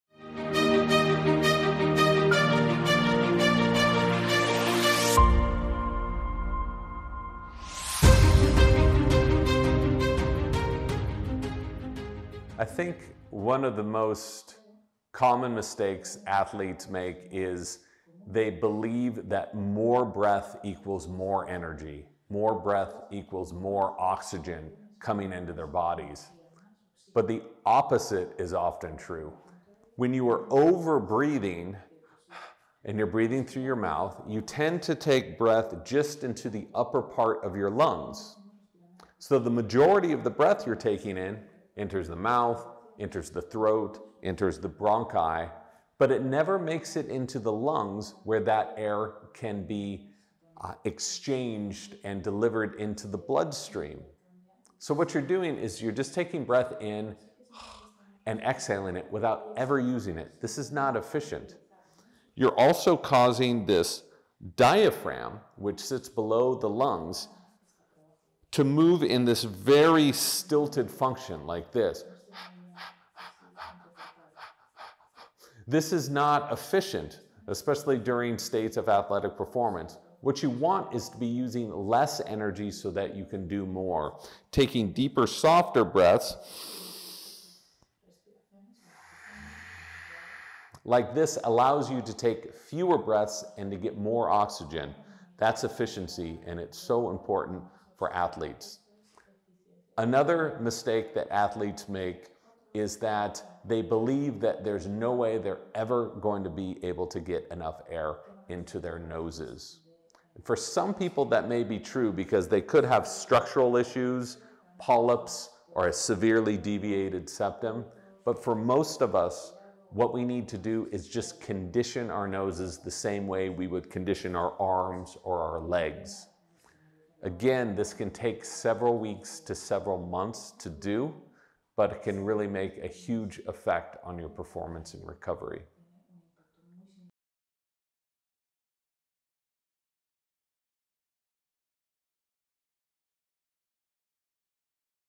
💫 Los ERRORES comunes en la RESPIRACIÓN - James Nestor | SPORT LIFE
Aprende a respirar correctamente con James Nestor, autor del libro "Respira".

En este segundo vídeo conocerás los 3 o 4 errores comunes a la hora de respirar correctamente.